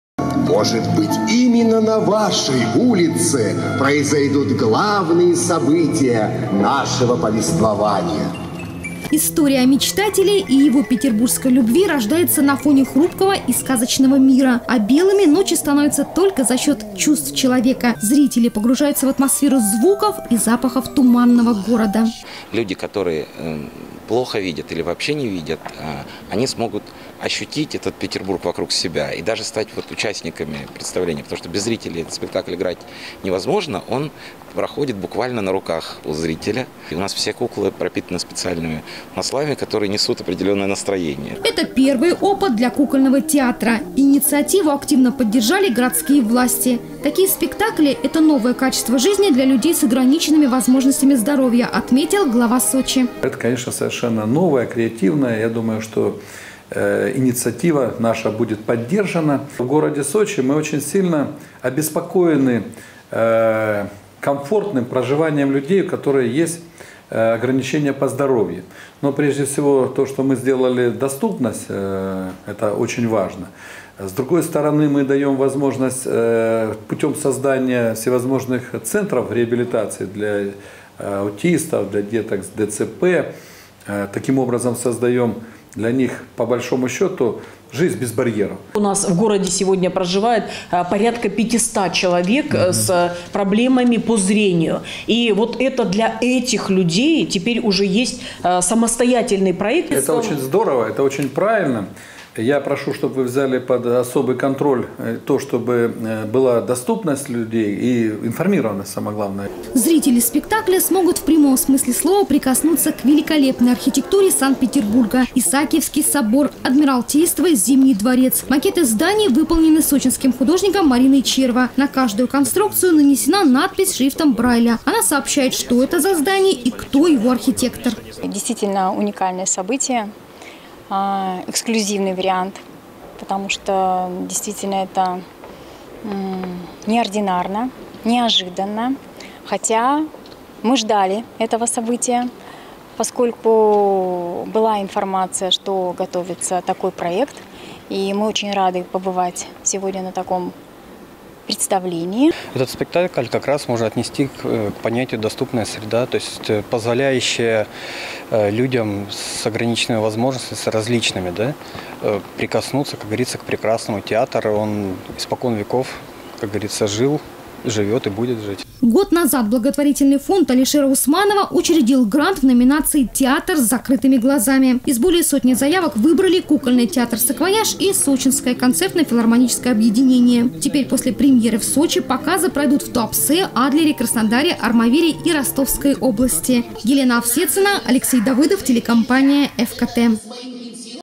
Спектакль в полной темноте показали в Сочи. Новости Эфкате
В Зимнем театре состоялась премьера «Белых ночей» Достоевского. Постановка подготовлена специально для людей с нарушением зрения.

История о Мечтателе и его петербургской любви рождается на фоне хрупкого и сказочного мира. Зал погружается в темноту. А белыми ночи становятся только за счёт чувств человека. Зрители погружаются в атмосферу звуков и запахов туманного города.